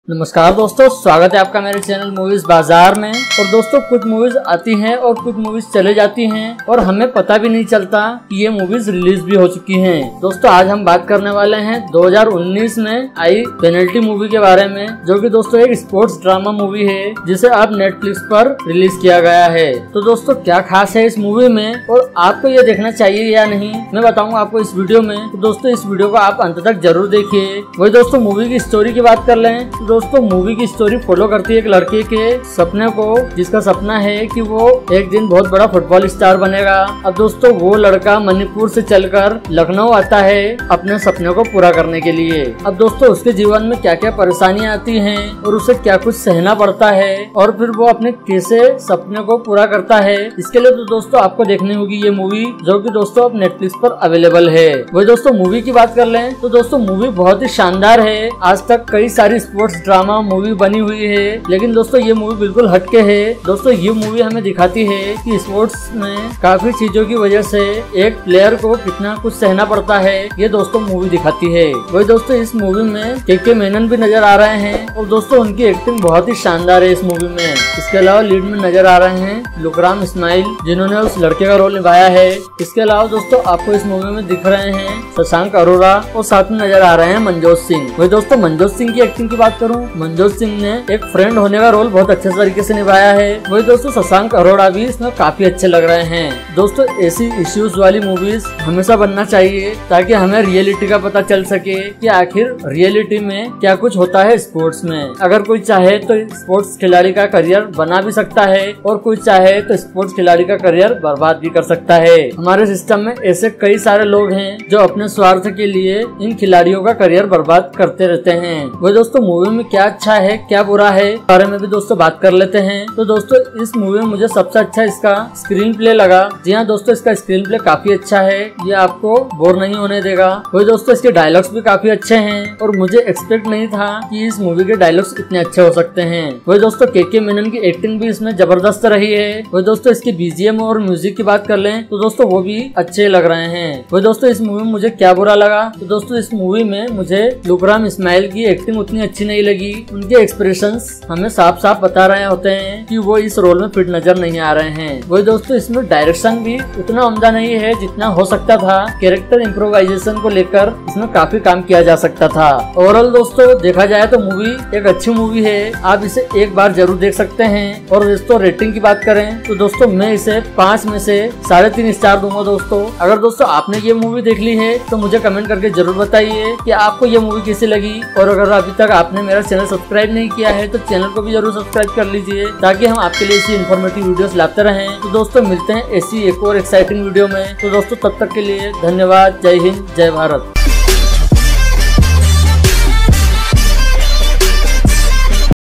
Netflix Penalty Movie Review | Lukram Smil | Kay Kay Menon | Shashank Arora
Well, the movie is based on a boy who hails from the North-Eastern part of India’s dreams to play football and the obstacles he has to cross to achieve it. The film covers all kinds of layers in one single film from negative backlashes, social pressures to a complete change over in one's life.